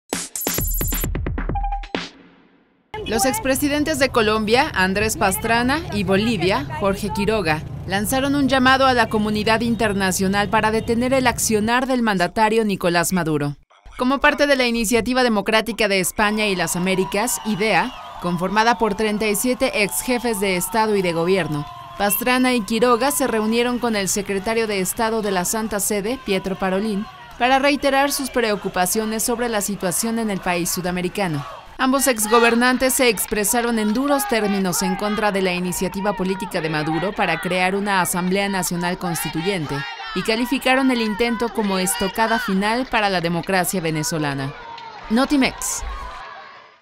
Lanzan llamado internacional para detener acciones de Nicolás Maduro
Tras reunirse con Parolin, los expresidentes advirtieron que si pasa la constituyente impulsada por Maduro, Venezuela se convertirá en la "Norcorea caribeña"